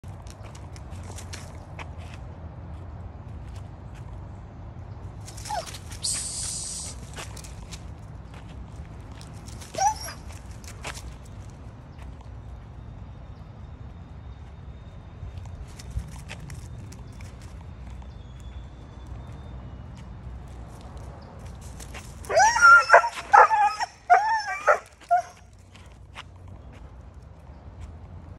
Doberman going insane over a Cat
Pepper is going crazy because she saw a cat😱